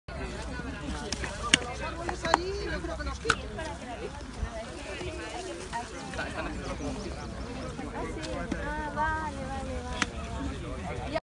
Vecinos plantan árboles como homenaje a Ámparo Pérez.